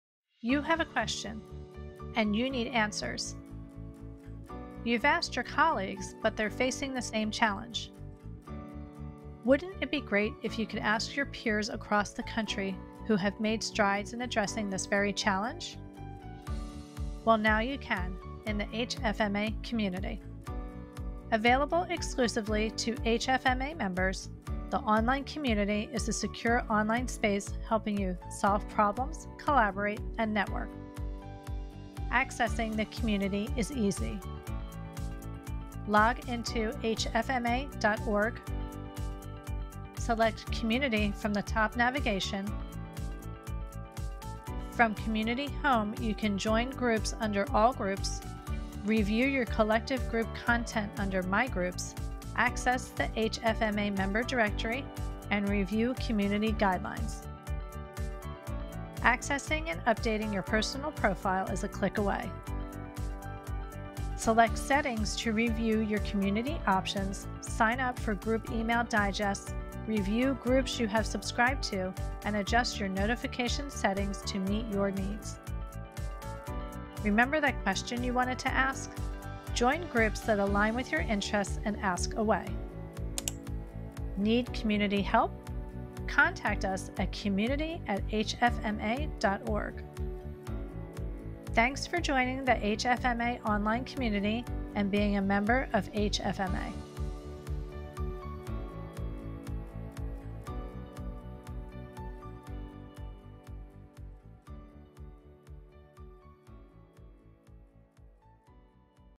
Welcome to the HFMA Community
Get connected to an interactive online community of HFMA members committed to helping their organizations – and each other – thrive. Solve problems, collaborate, network. It’s never been easier.